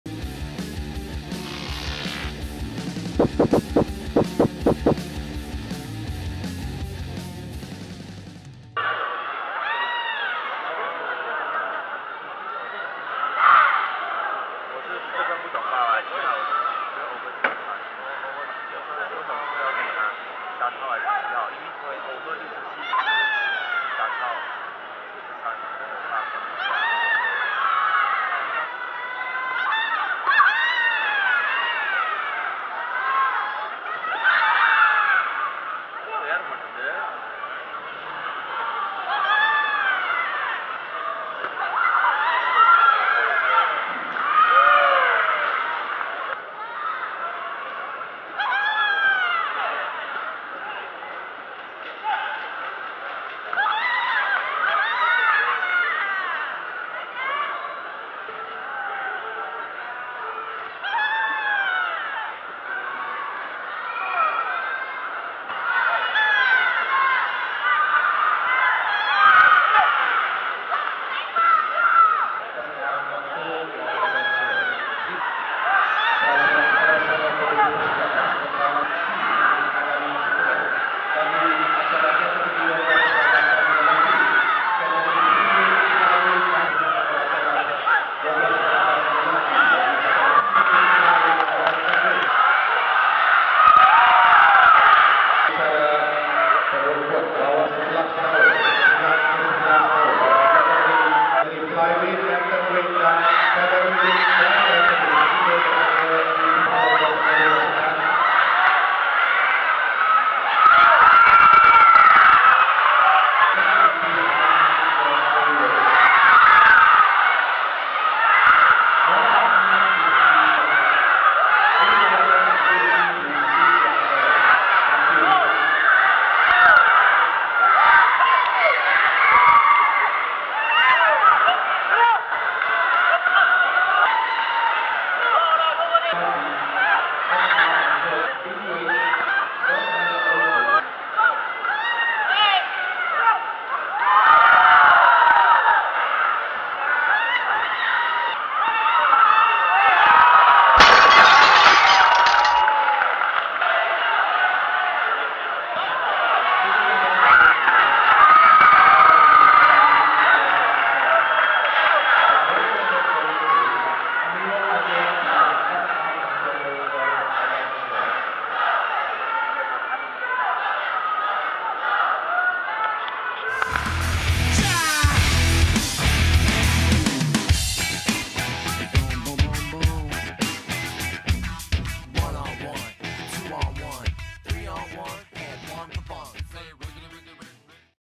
Shakila Dharshini (RSB) VS Nur Fasha Qistina (MATSA) - 5th TM(WTF)-NSC-MILO
Shakila Dharshini (RSB) VS Nur Fasha Qistina (MATSA)
open female below 46kg final

5th TM(WTF)-NSC-MILO National Interclubs Taekwondo Championship